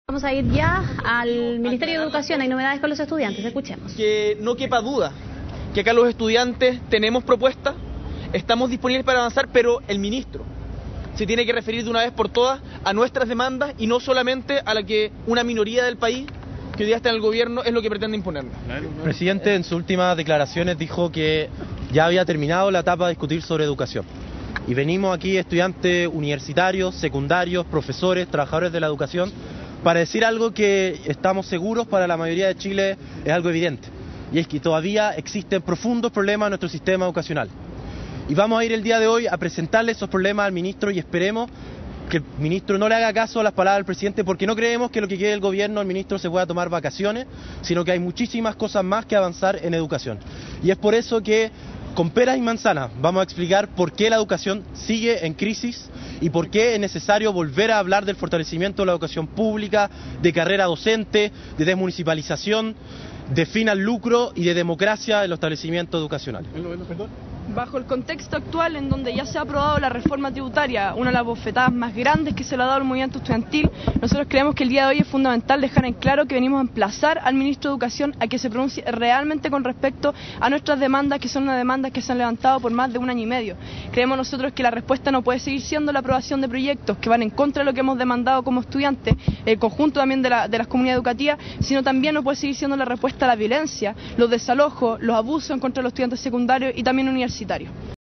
Estudiantes llegan al Mineduc para reclamar falta de diálogo del ministro Beyer
Le plantearán sus demandas al jefe de la cartera de Educación.